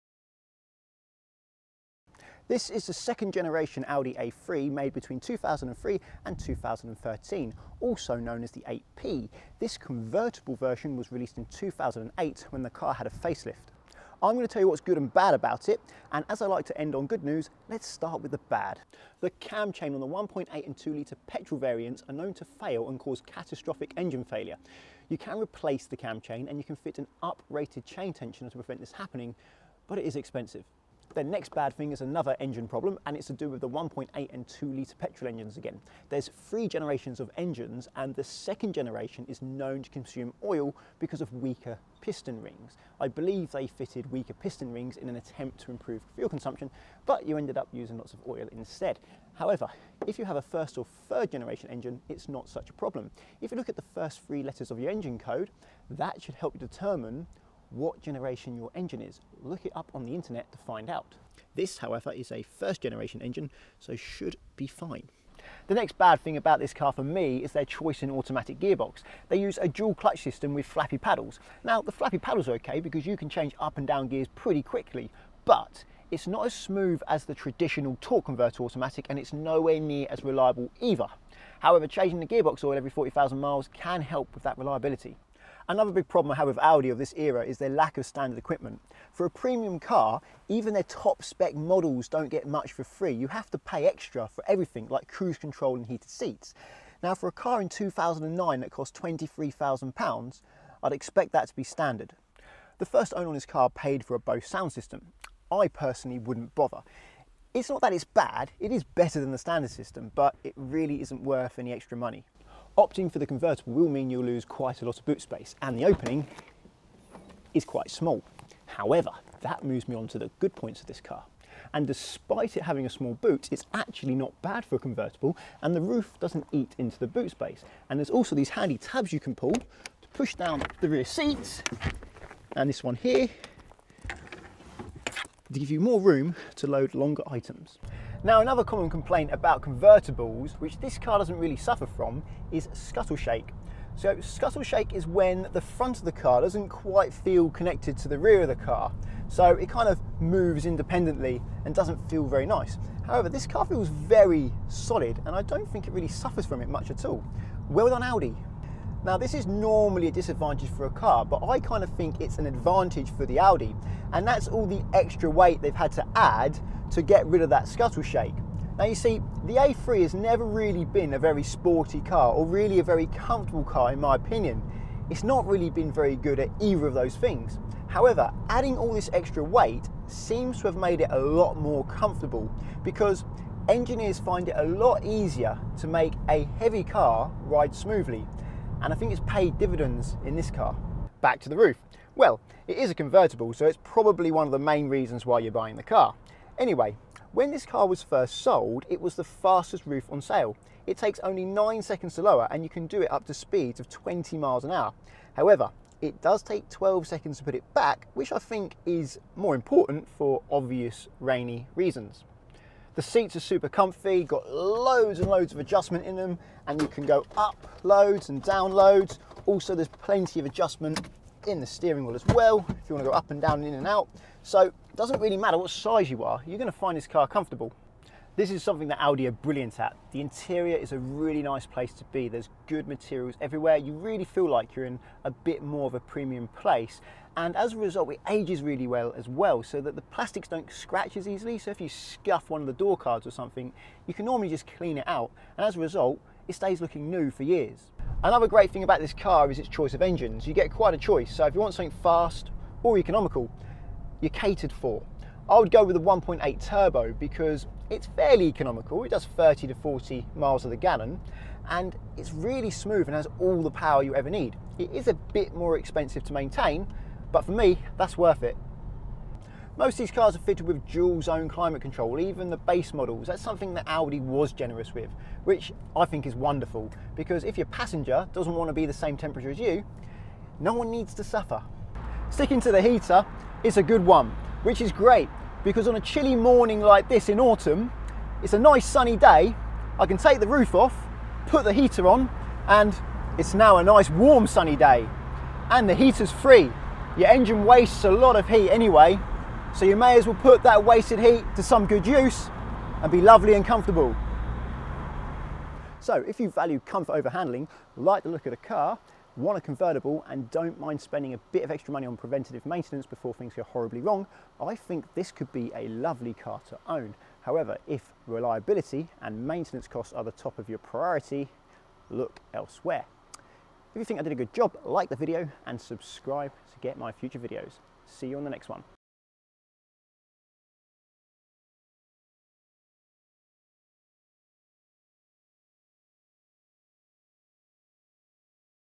The good and the bad about the Audi A3 (8P) Cabriolet 2008-2013
The good and the bad about the Audi A3 cabriolet made between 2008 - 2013.

This second generation A3 which actually went on sale in 2003 received a face lift in 2008 which included the launch of this convertible version. For the geeks out there the second generation is officially known as the 8P.

Audi claimed that the convertible was supposed to be as good to drive as the hard top and had the fastest roof on sale going from fully closed to fully open in only 9 seconds. At the time motoring journalist Tiff Needell, who drove the car for the TV series Fifth Gear disagreed with Audi's claim saying, "if you hit a big bump, the steering wheel shudders and the glove box creaks."

This particular A3 is the sport model with a 160BHP 1.8 EA888 turbo petrol engine mated to a dual clutch DSG gearbox and was first registered in March 2009.